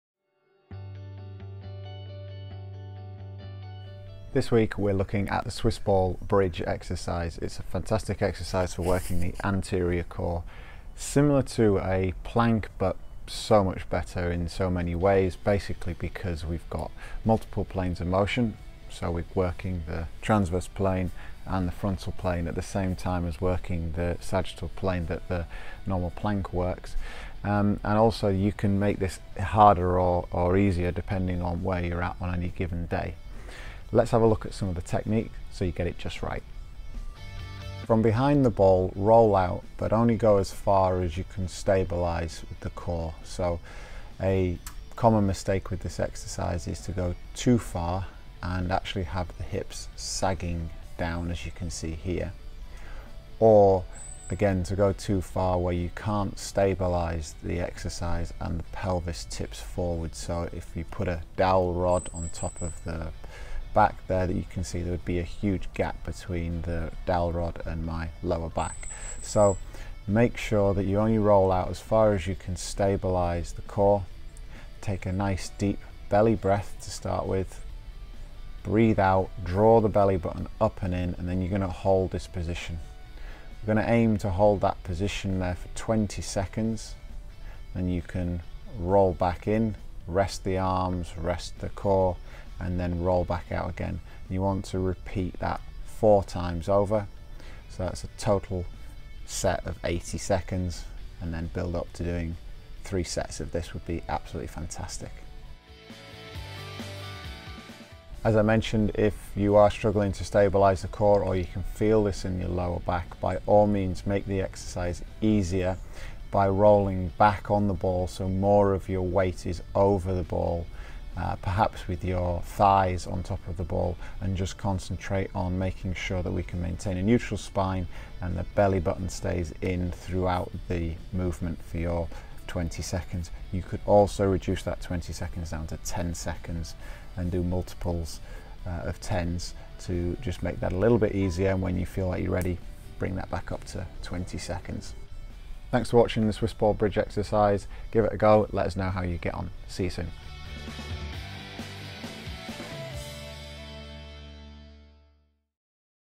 Swiss Ball Prone Bridge | How To...
Swiss Ball Prone Bridge

Abs of Steal :-)

The plank is an overused, misunderstood and poorly executed exercise, in my opinion.  It creates excessive tension in the six pack muscle (rectus abdominus) and therefore increases the curvature of the upper body.  This leads to bad shoulders, back pain and neck problems, to name just a few.

The prone bridge can be a nice alternative because it works the same muscles but you can vary the intensity more easily depending on your needs/ability and it also incorporates other planes of motion.  Therefore there is more going on.